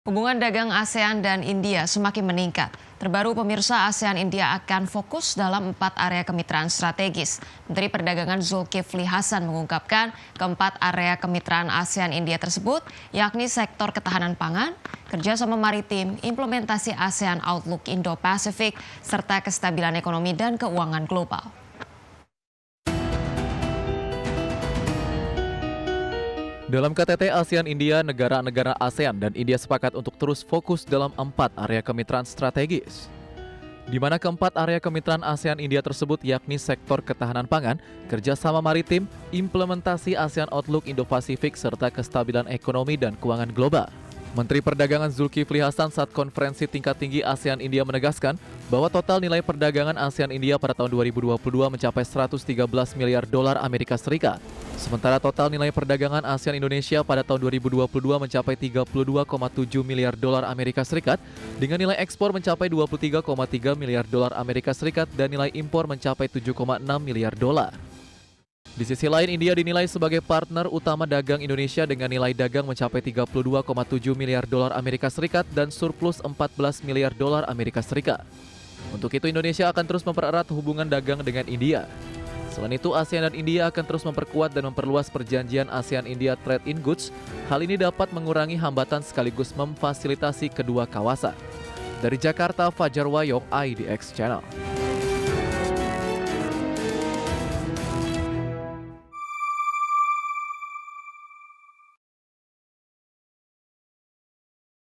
Nilai Dagang RI-INDIA Surplus USD14 Miliar | IDX CHANNEL
Hubungan dagang ASEAN dan India semakin meningkat. ASEAN - India akan fokus dalam empat area kemitraan strategis. Menteri Perdagangan, Zulkifli Hasan, mengungkapkan keempat area kemitraan ASEAN - India tersebut yakni sektor ketahanan pangan, kerja sama maritim, Implementasi ASEAN Outlook on the Indo-Pacific (AOIP) serta kestabilan ekonomi dan keuangan global.